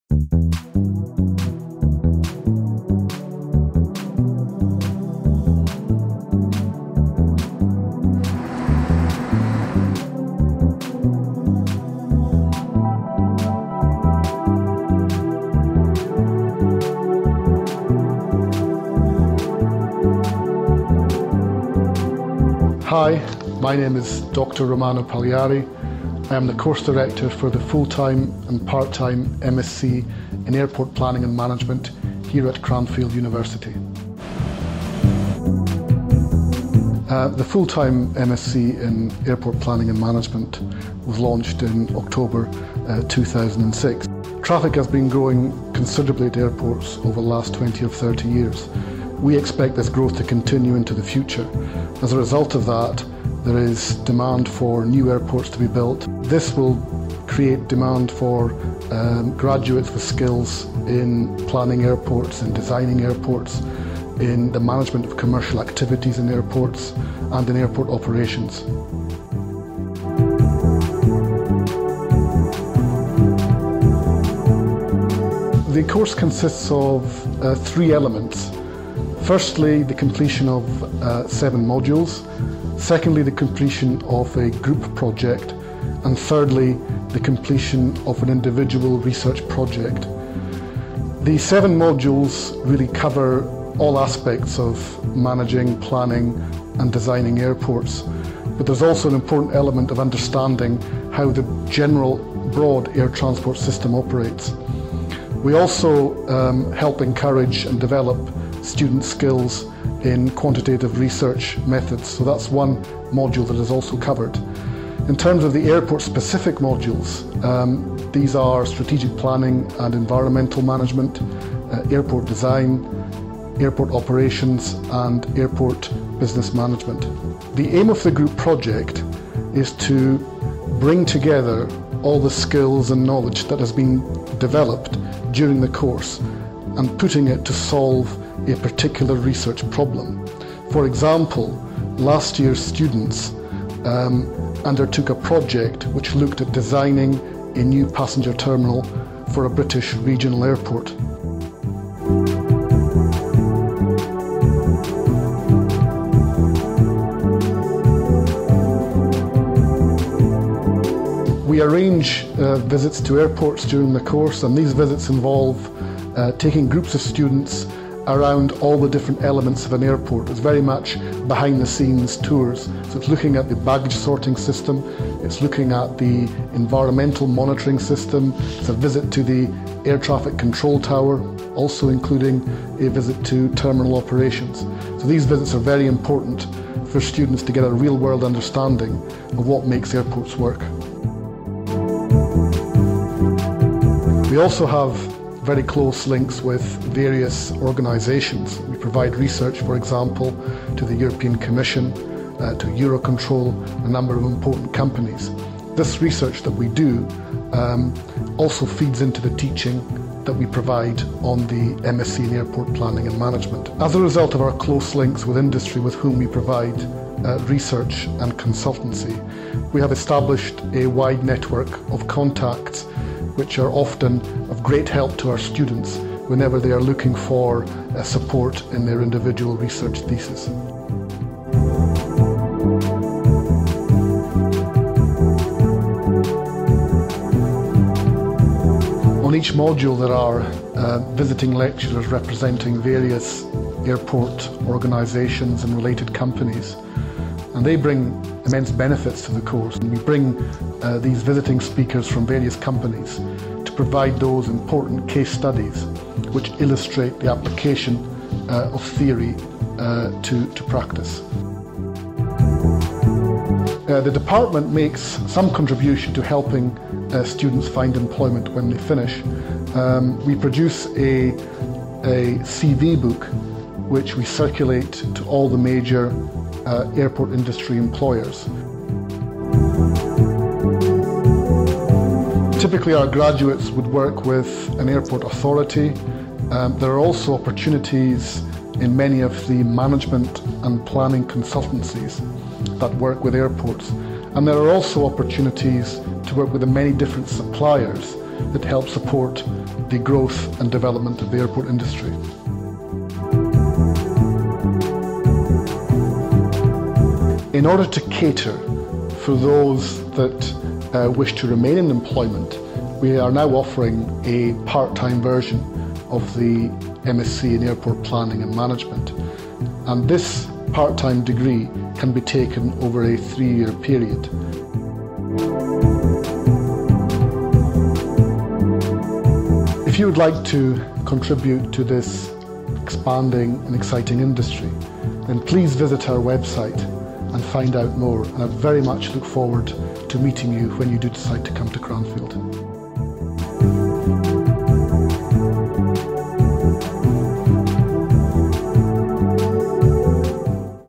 Course video - Airport Planning and Management MSc at Cranfield University
The course director and students describe the Airport Planning and Management MSc at Cranfield University